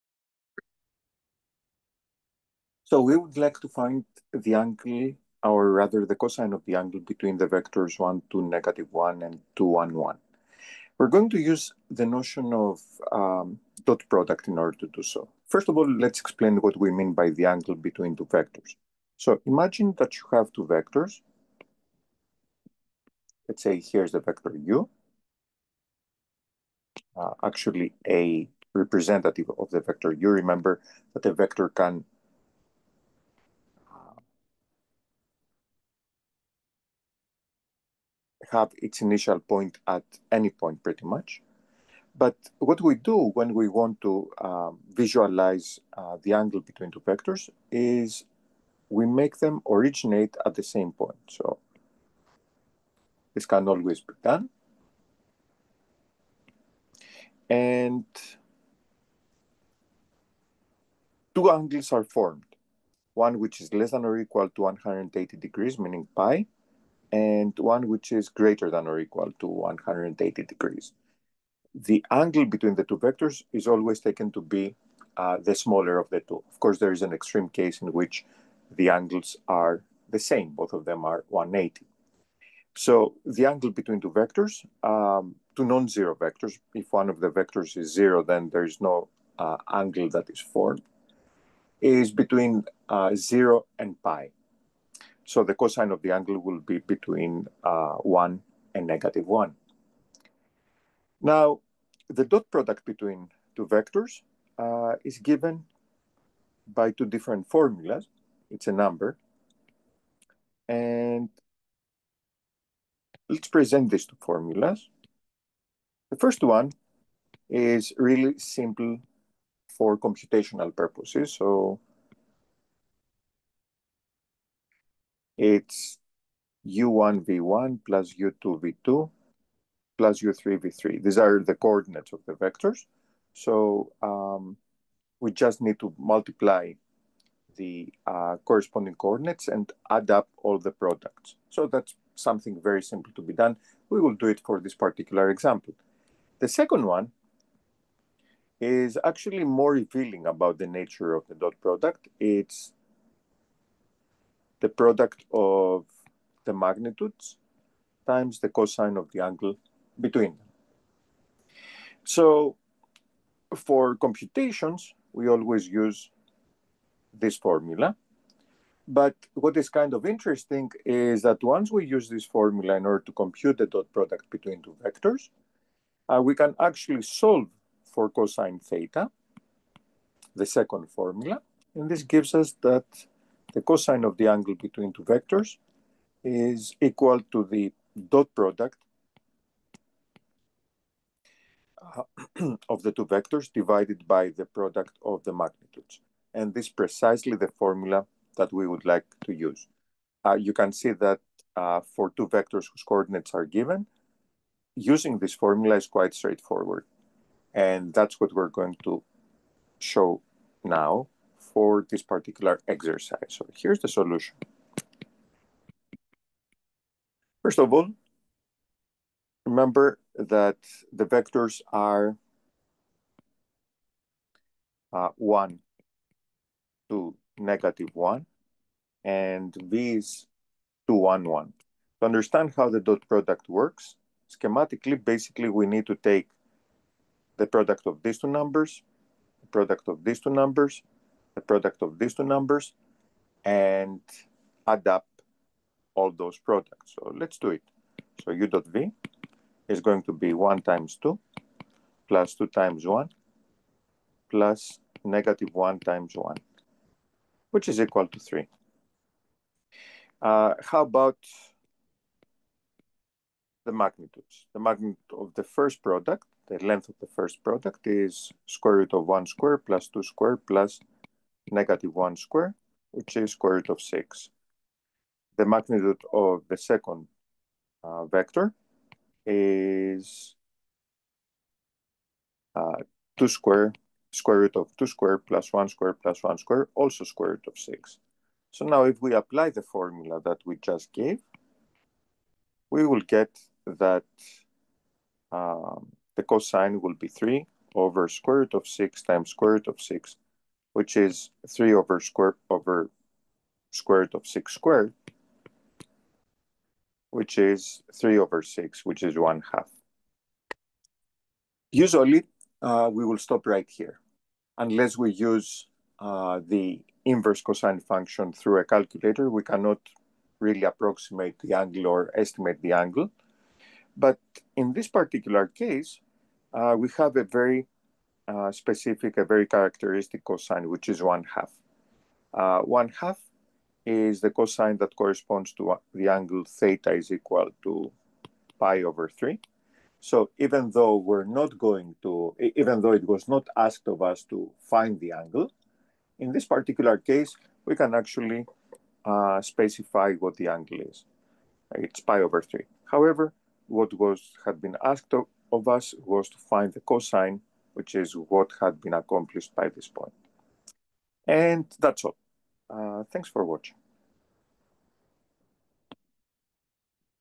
Calculus III - Problem of the day - 04 - Cosine Of Angle Between Two Vectors
In this video, we use the dot product of two vectors in order to compute the cosine of their angle.